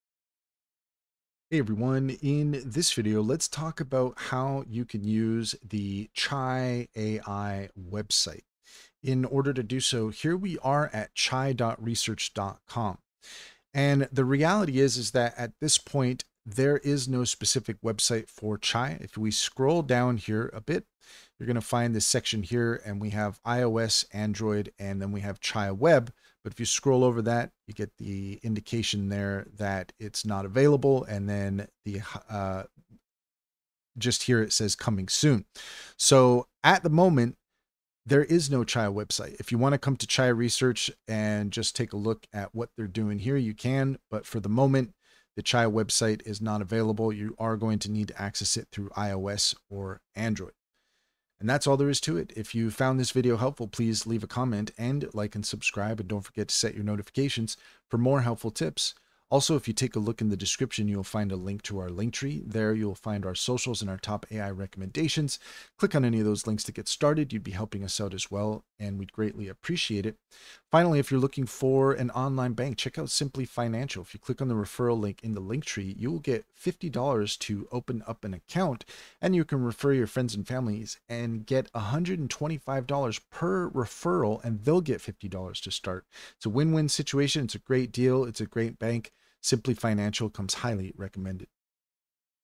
How To Use Chai Ai Website – Ultimate Guide
Discover how to navigate and maximize the features of Chai Ai effectively.

🚀 AI Tools, Discounts & Webinars – All in One Place!